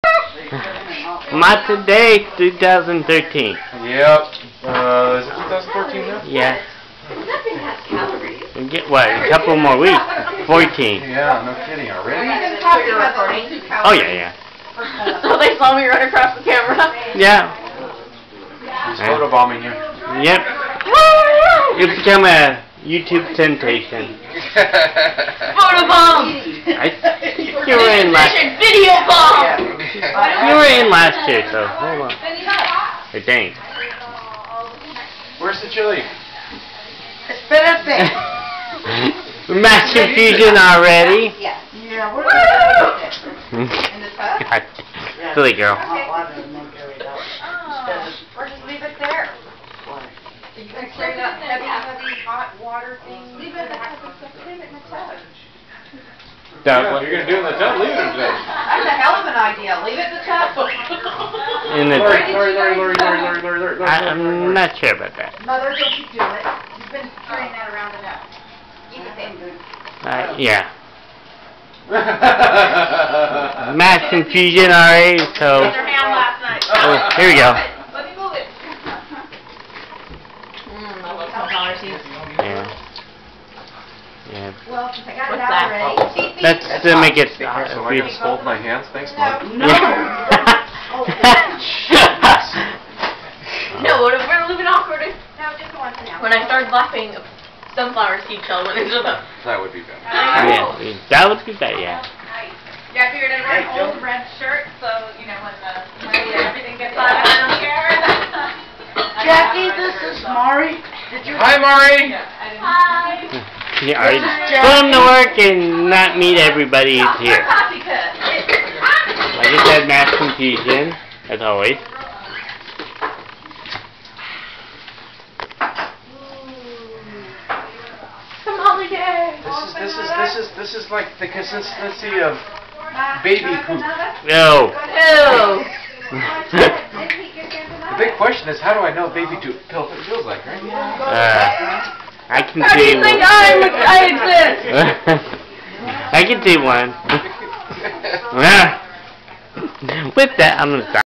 Tamales Day 2013 part 1
This Tamles Day 2013 we do this once a year and we made least 30 dozon